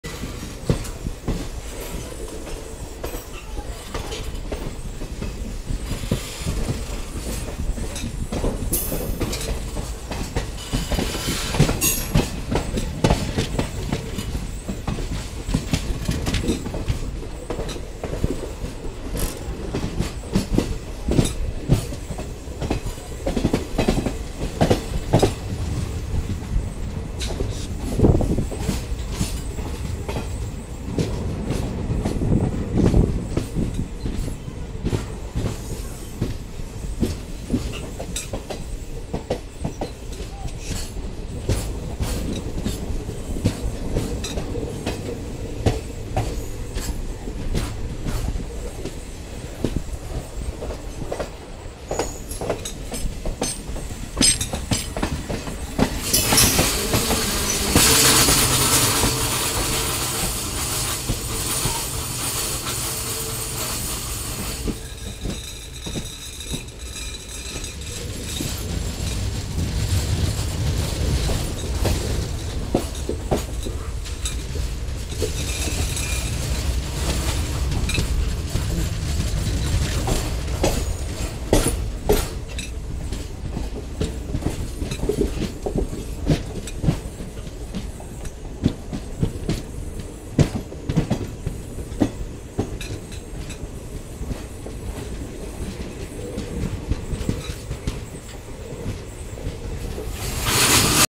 Track sounds @ Chitradurga
Bengaluru city - Hosapete Passenger arriving at Chitradurga.